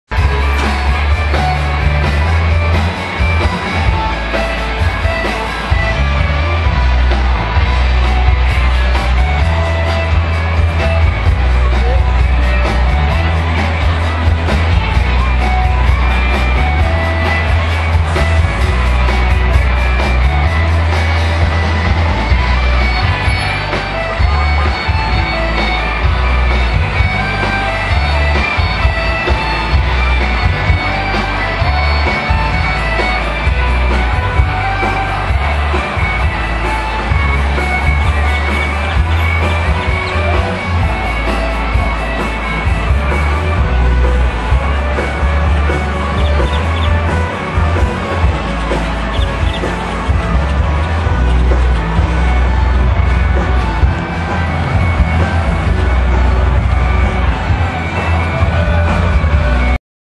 St. Patrick's Day 2010. McKinney Avenue.Dallas, Texas. 3 of 5. A very short clip.
Showing some of the St. Patrick's Day activities on McKinney Avenue in Dallas, Texas. March 17, 2010.
A short minute clip.

 3rd of five videos..